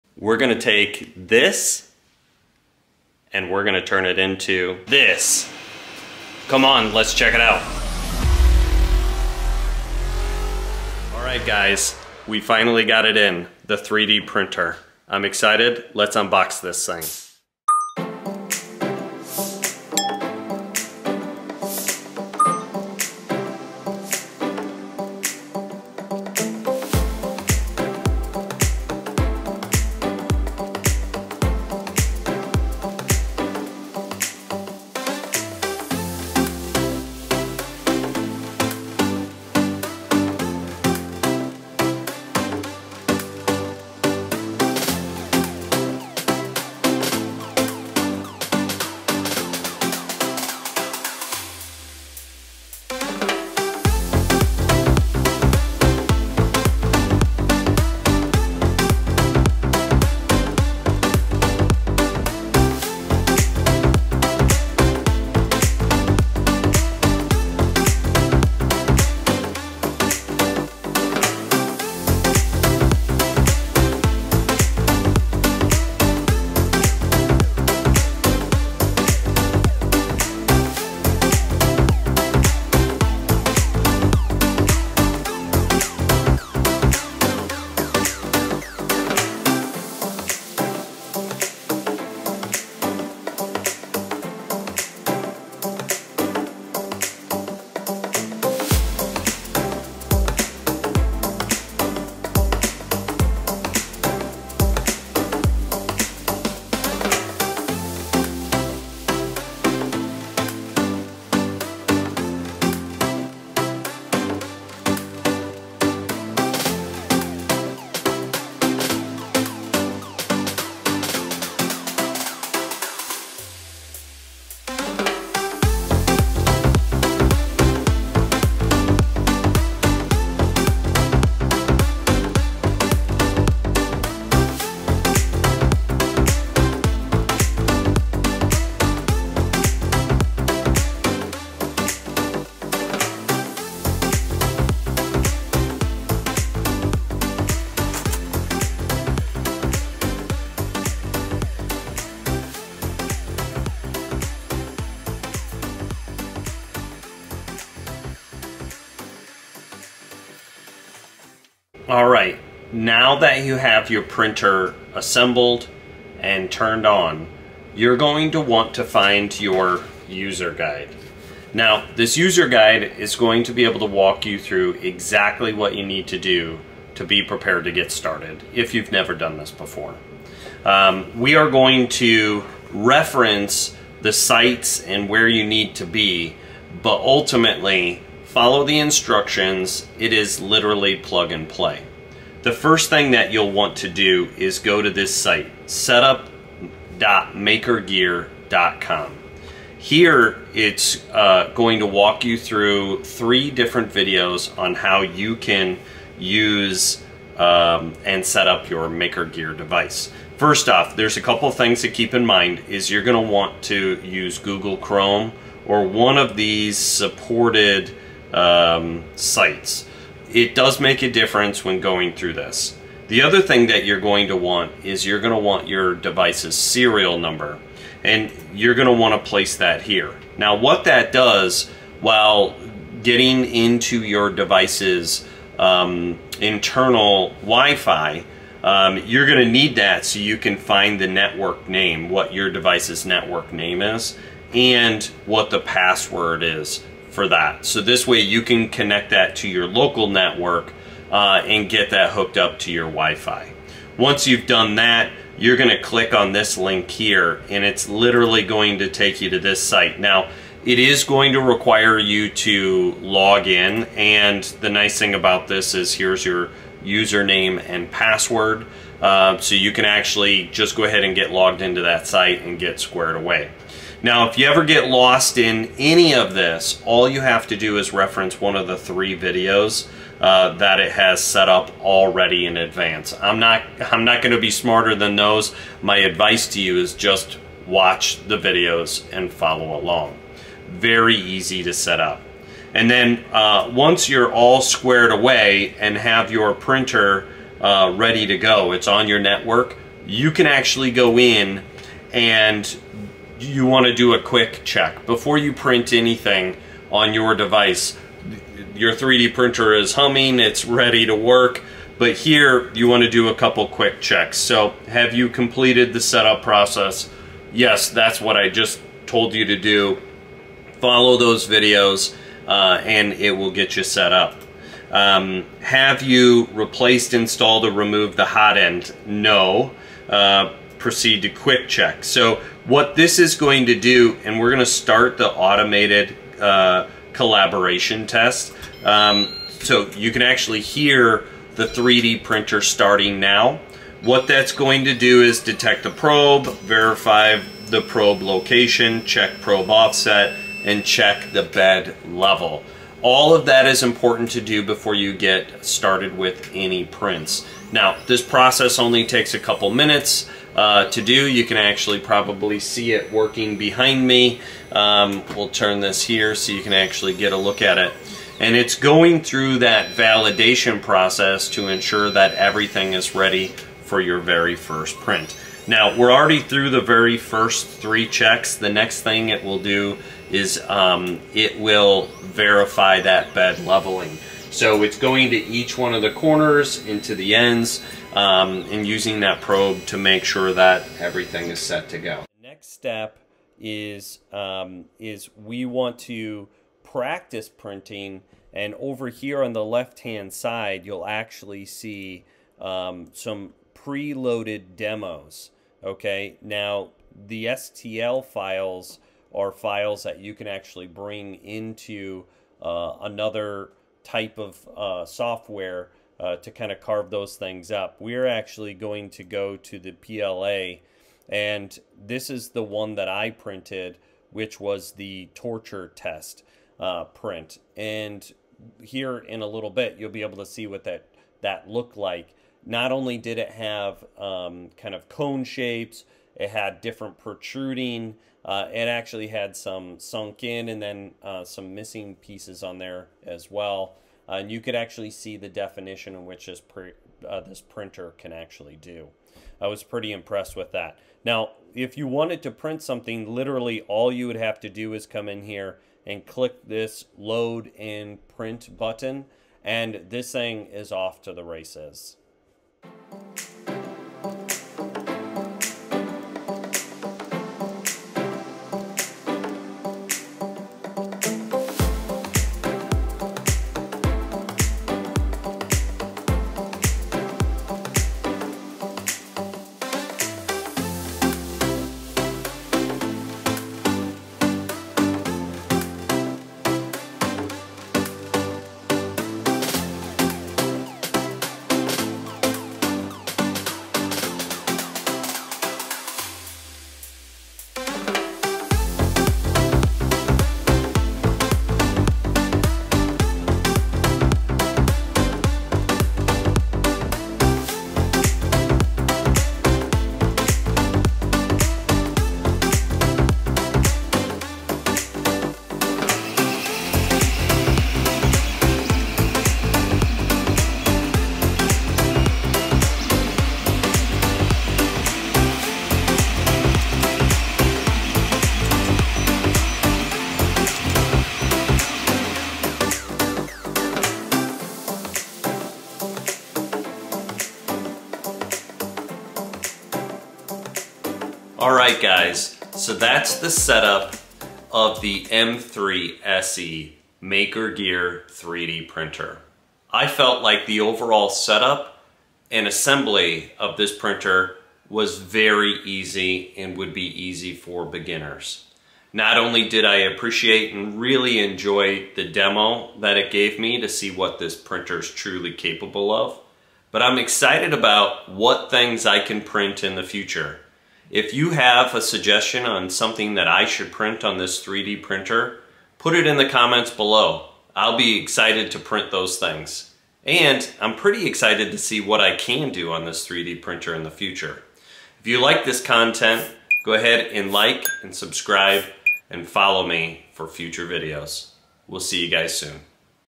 MakerGear M3 SE 3D Printer Setup and First Print
I finally received my MakerGear M3 printer and couldn't wait to get it out of the box and put it together.  Please watch a step by step on what to do when you get your first printer.

I purchased this Makergear Printer.